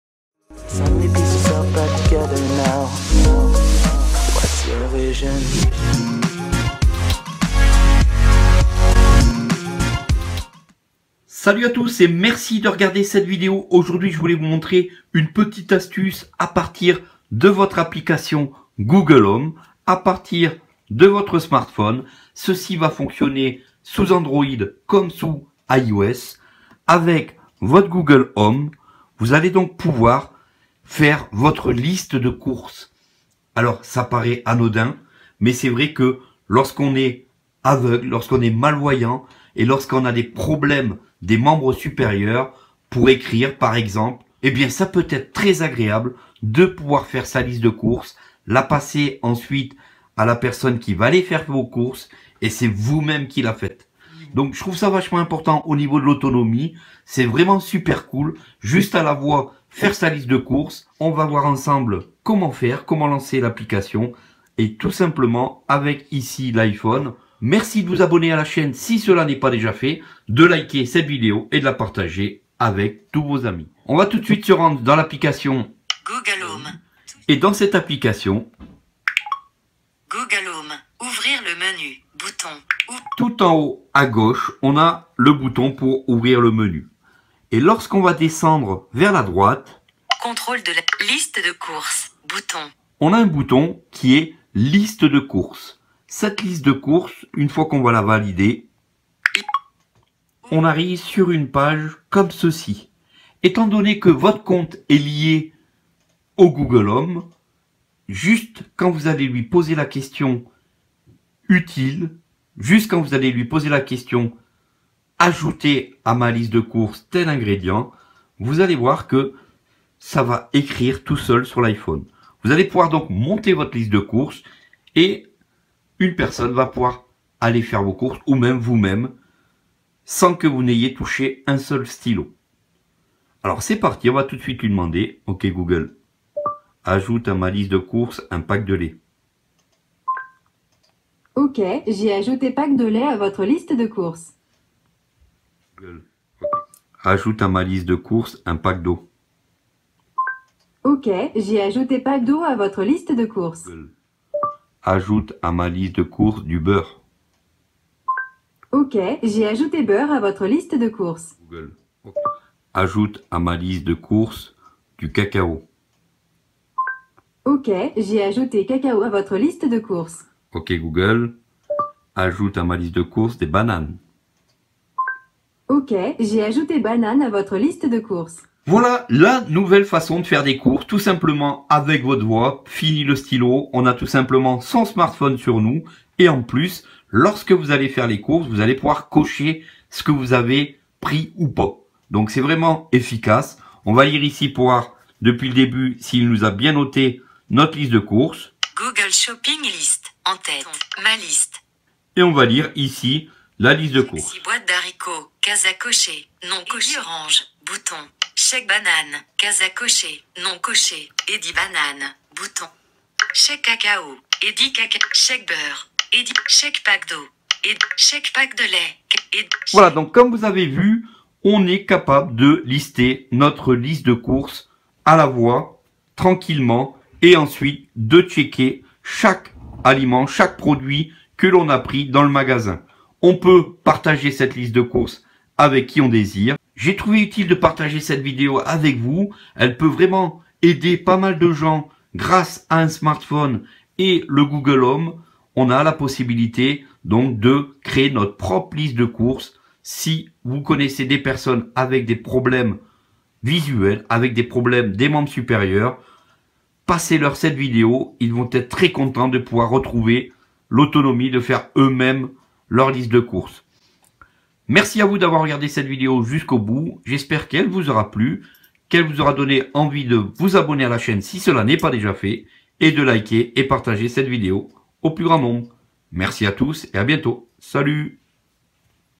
Google Home Test Application Liste De Courses Sur Android Et iOS !
Salut sur Google Home Enormément de choses sont possible en voici encore une autre qui peux vraiment faciliter la vie de certaines personnes. l'application liste de course va vous faire gagner beaucoup de temps dans cette vidéo merci de vous aboner et à bientôt pour une prochaine.